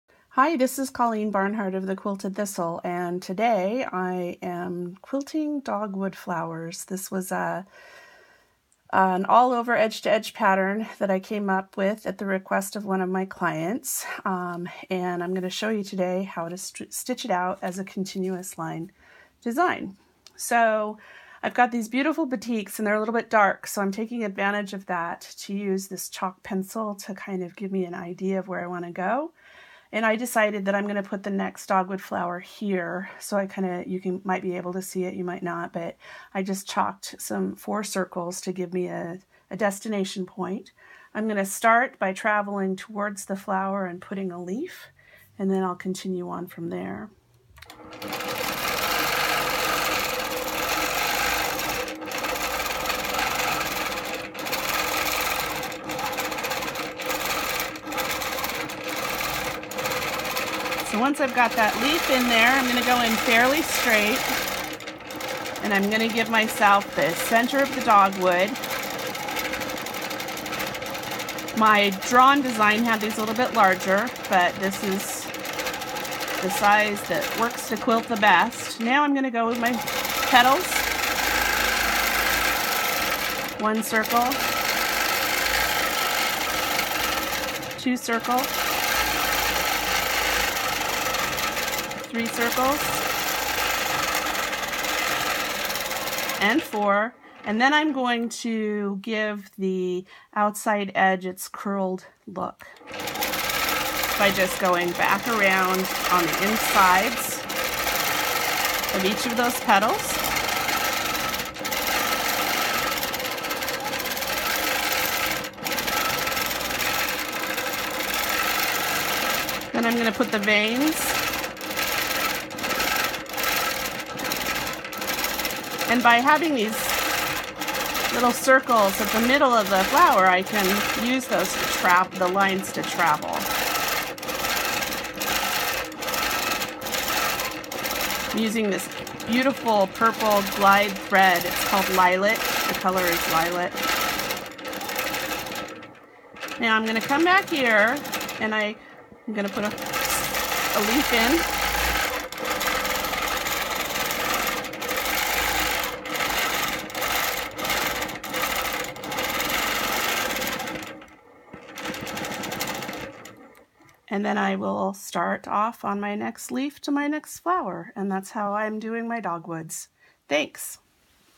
Dogwood Flower Quilting Motif by Coleen Barnhardt of The Quilted Thistle
Watch Coleen Barnhardt of The Quilted Thistle freemotion quilt a dogwood flower design she designed as an edge-to-edge motif using her Innova longarm quilting machine.